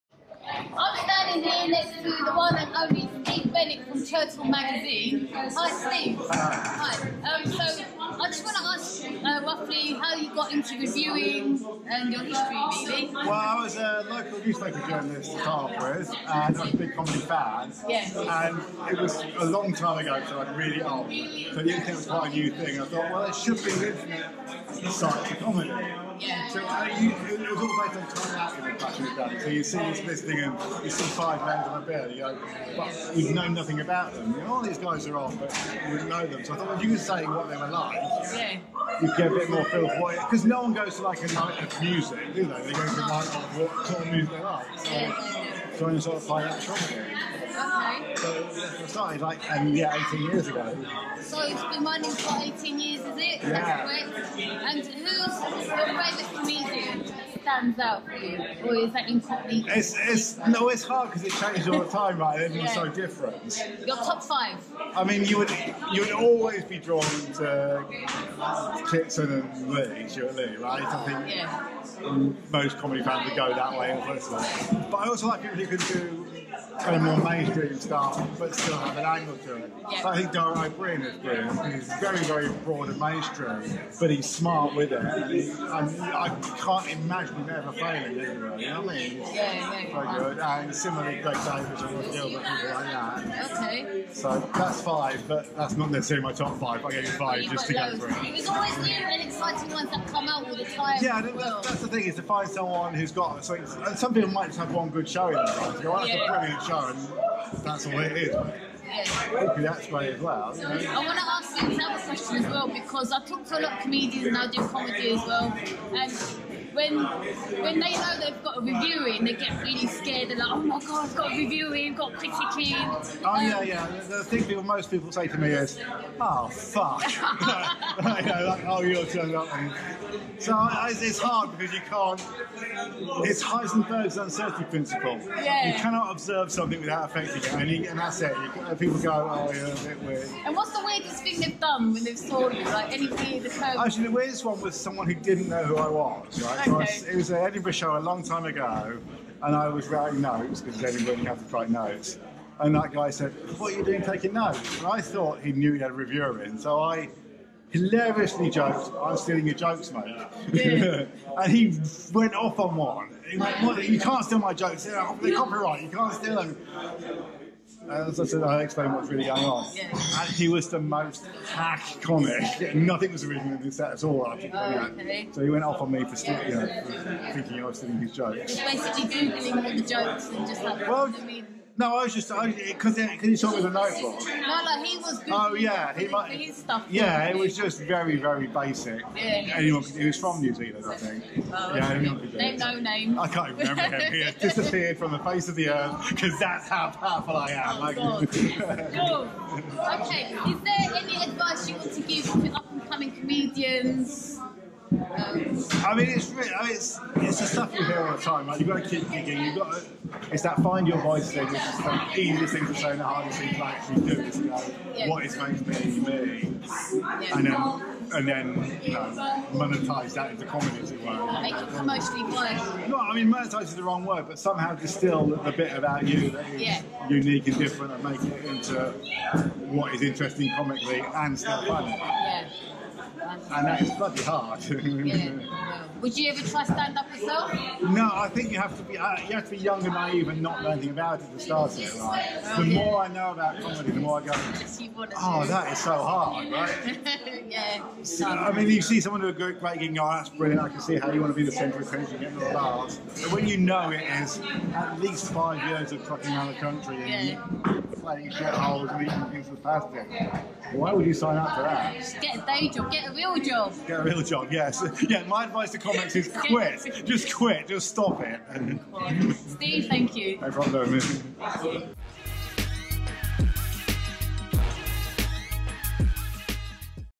®️ The Funny Circle - Comedy Vlog l Watch Steve Bennett Interview Only on ®️ The Funny Circle
We caught up with Steve Bennett of Chortle.co.uk at the Eastern European Comedy Festival. Comedian Narin Oz chats with Steve about his favourite comedians.